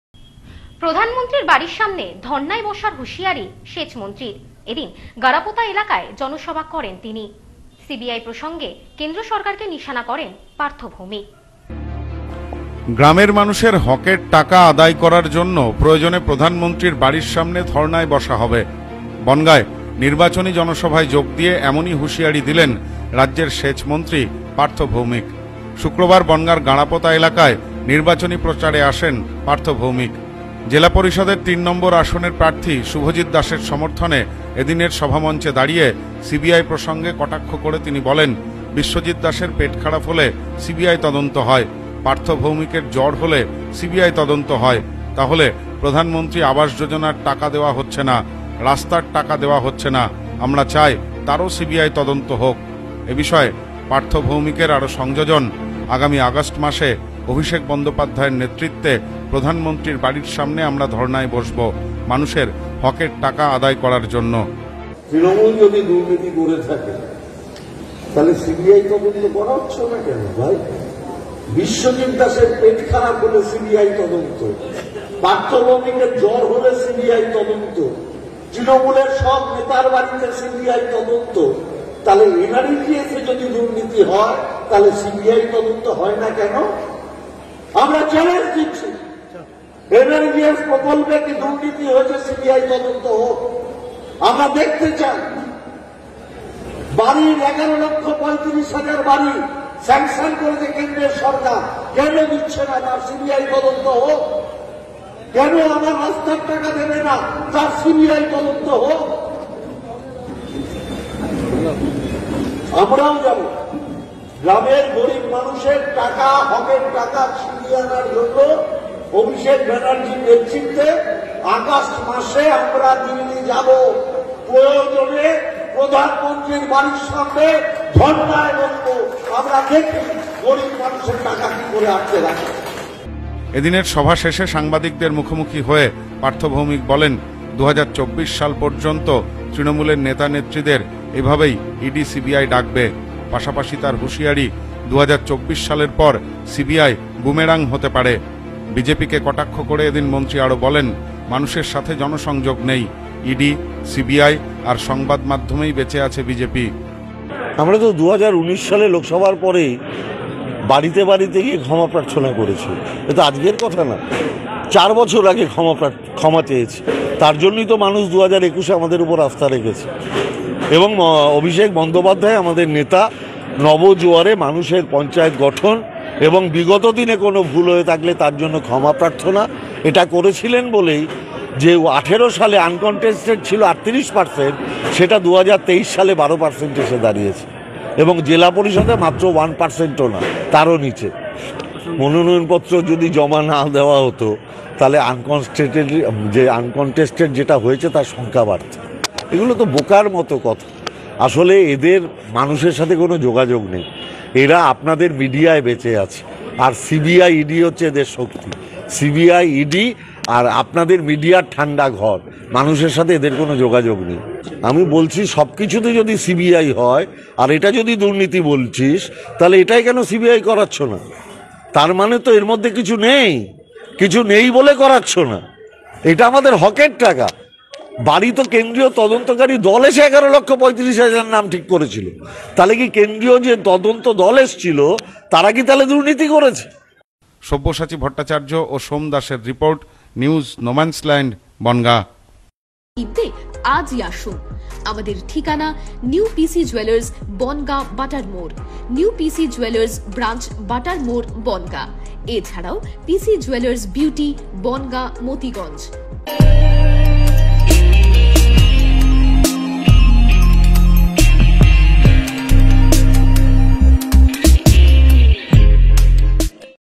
বনগাঁয় এসে প্রধানমন্ত্রীর বাড়ির সামনে ধর্নায় বসার হুঁশিয়ারি সেচ মন্ত্রীর।
বনগাঁয় এসে প্রধানমন্ত্রীর বাড়ির সামনে ধর্নায় বসার হুঁশিয়ারি সেচ মন্ত্রীর। এদিন গাঁড়াপোতা এলাকায় জনসভা করেন তিনি। সিবিআই প্রসঙ্গে কেন্দ্র সরকার কে নিশানা করেন পার্থ ভৌমিক।